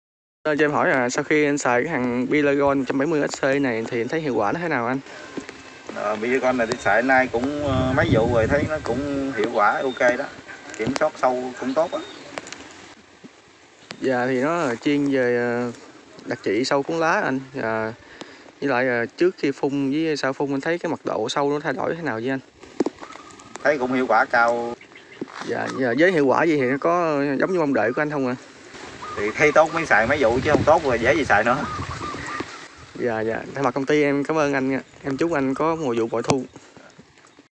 Đặc trị sâu cuốn lá với loại thuốc này - Nông dược Việt Nam VN-APC
Anh Sanh ở Đồng Tháp đã tin tưởng sử dụng Pylagold 170SC để trừ sâu cuốn lá, mời bạn cùng trải nghiệm hiệu quả rất tốt của sản phẩm này !

Pylagold 170SC là thuốc được hỗn hợp bởi 2 hoạt chất có tính tiếp xúc, vị độc, thấm sâu cực mạnh, hiệu lực kéo dài. Trừ được nhiều loại sâu miệng nhai và chích hút đã kháng thuốc.

Người thực hiện video: Phan Thanh Tấn.

𝐂𝐨̂𝐧𝐠 𝐭𝐲 𝐂𝐨̂̉ 𝐩𝐡𝐚̂̀𝐧 𝐍𝐨̂𝐧𝐠 𝐝𝐮̛𝐨̛̣𝐜 𝐕𝐢𝐞̣̂𝐭 𝐍𝐚𝐦 𝐕𝐍-𝐀𝐏𝐂
Địa chỉ công ty: Số 17/71 Phố Đỗ Quang, Trung Hòa, Cầu Giấy, Hà Nội